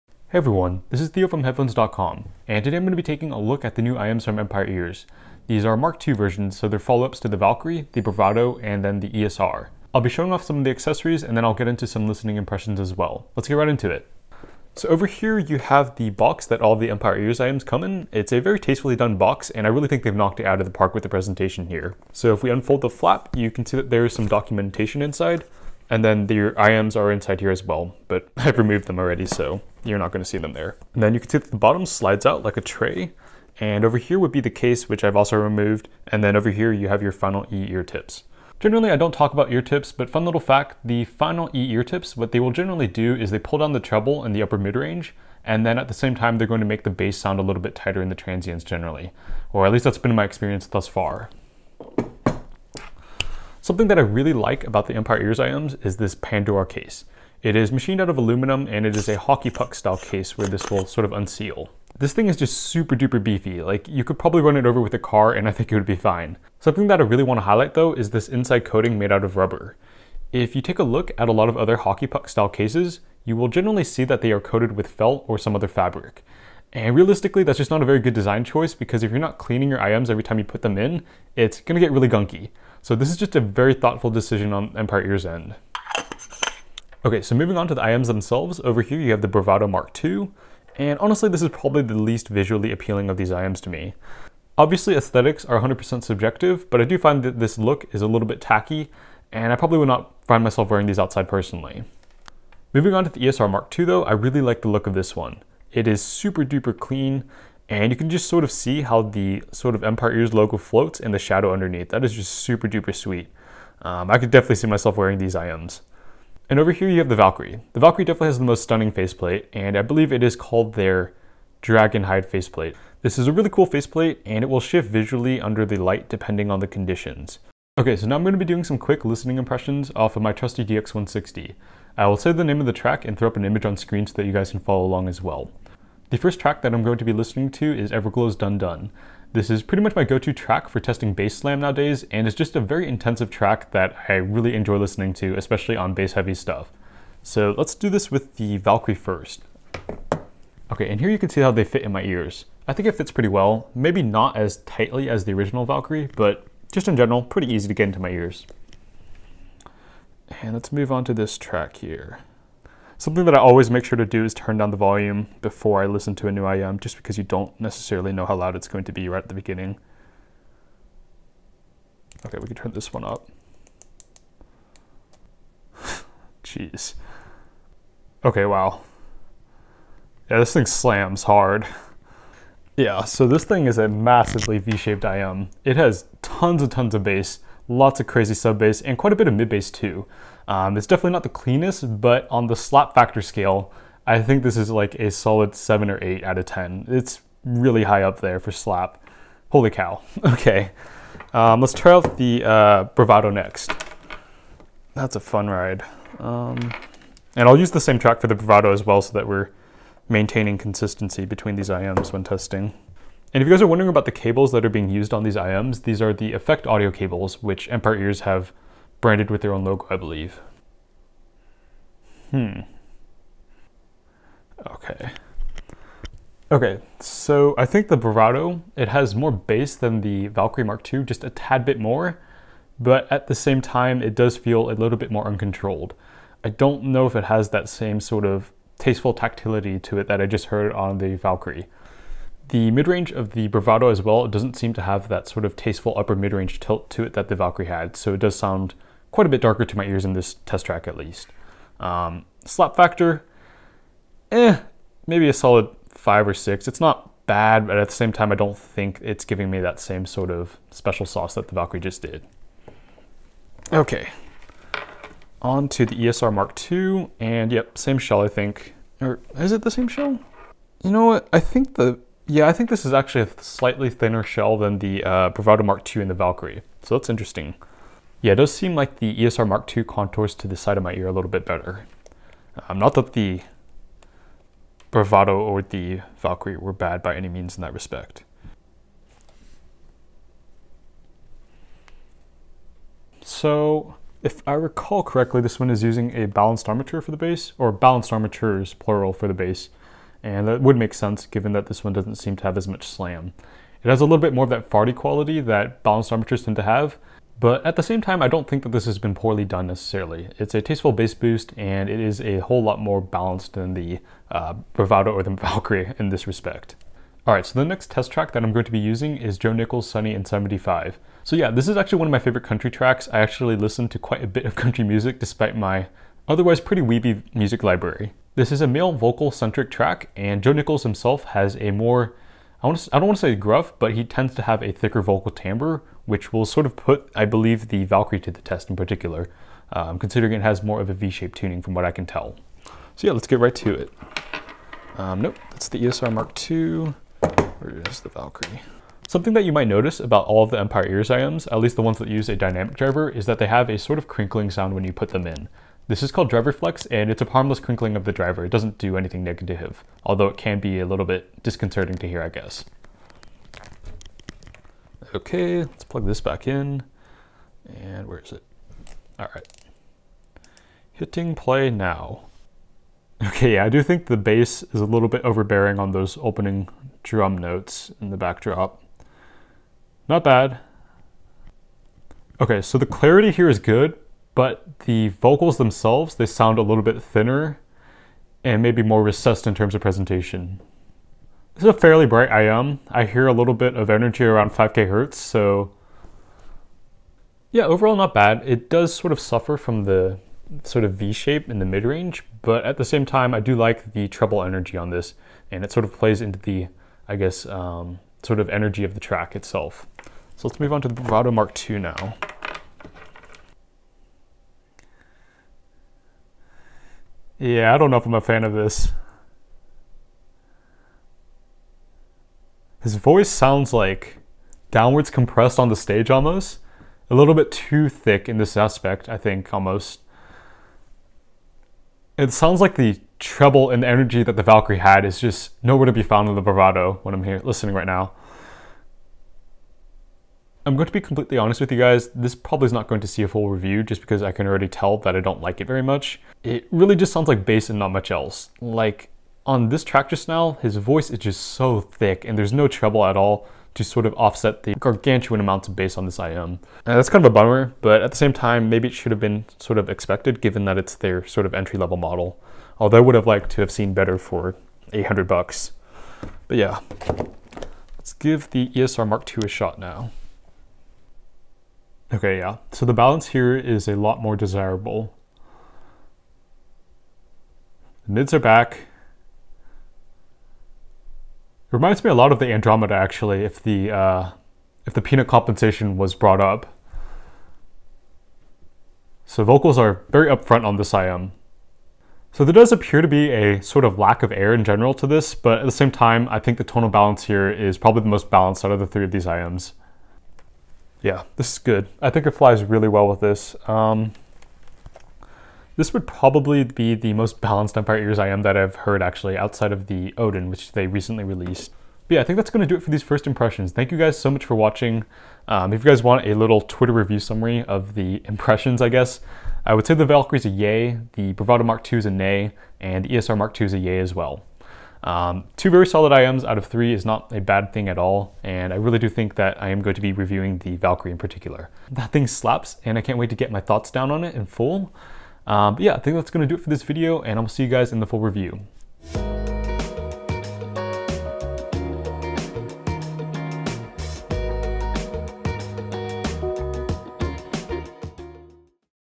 Empire Ears 2021 Lineup Refresh First Impressions - Bravado, ESR, Valkyrie Mk II Versions
0:00 Intro & Tangibles
2:16 Test Track #1
5:17 Test Track #2
8:29 Wrap-Up